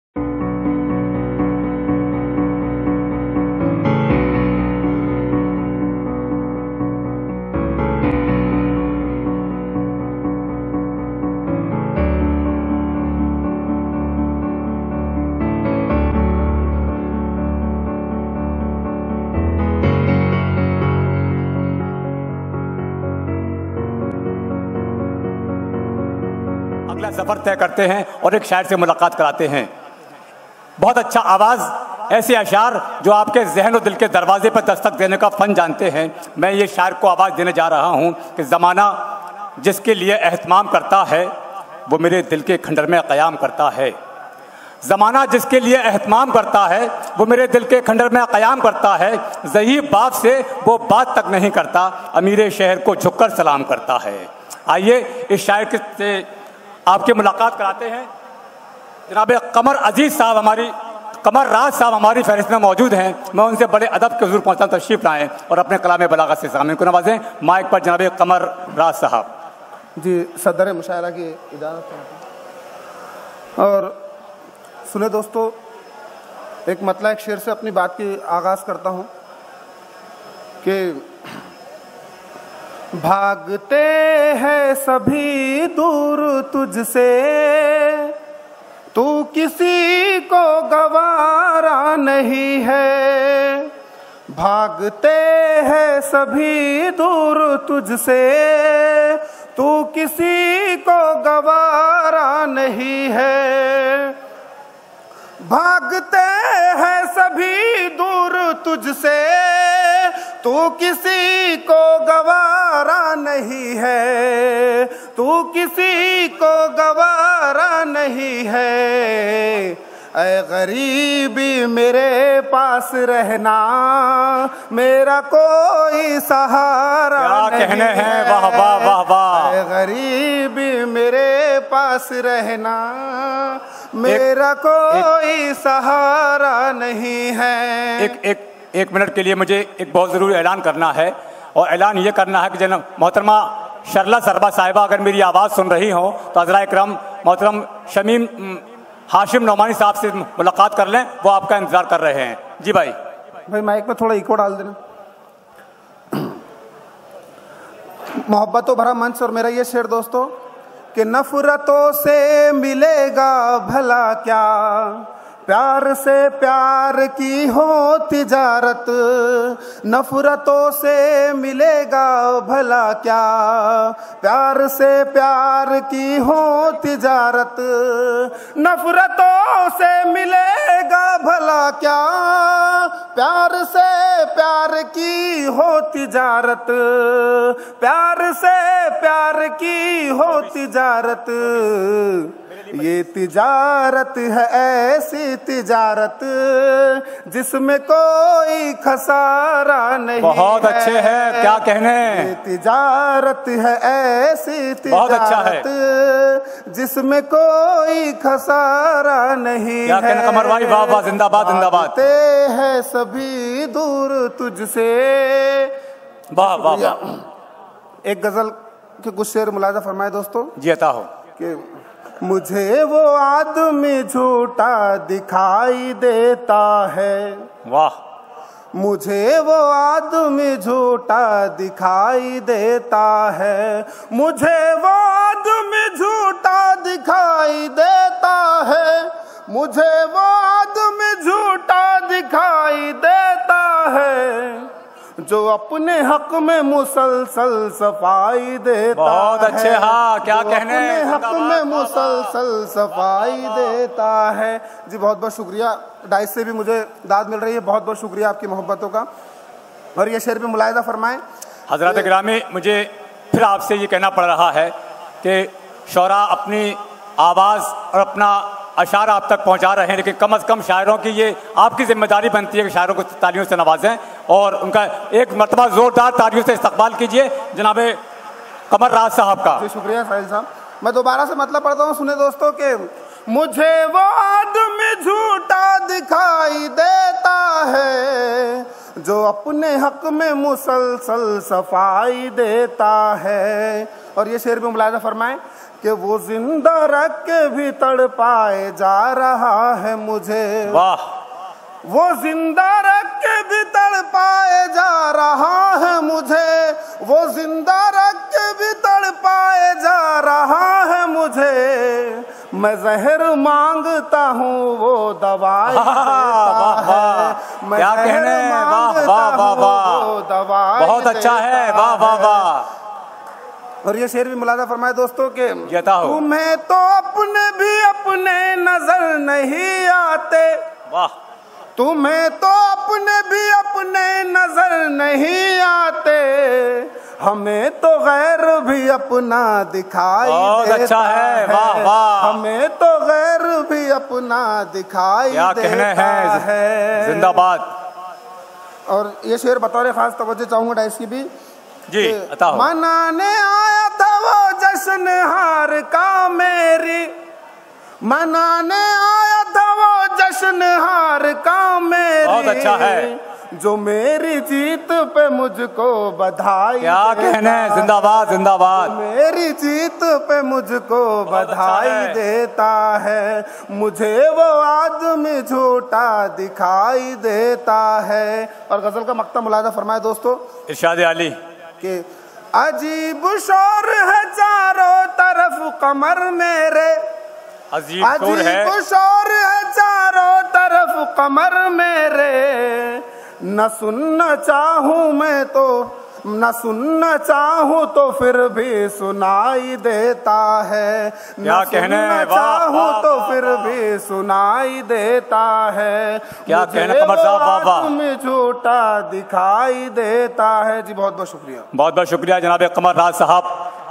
QAMAR RAAZ, Marathon Mushaira With NADEEM FARRUKH, Session 4 , MEDIA MARATHON
MEDIA MARATHON, MARATHON MUSHAIRA - Aye Watan Tere Liye
Garvit Computer Education And Social Welfare Trust
Presents MARATHON MUSHAIRA & KAVI SAMMELAN
A Great Literary Event Of 101:21Hrs
Creating HIstory And Making The World Record
Held At GARVIT CAMPUS, Kot Gaon, Near Toll Plaza Dadri, Goutam Budh Nagar, U.P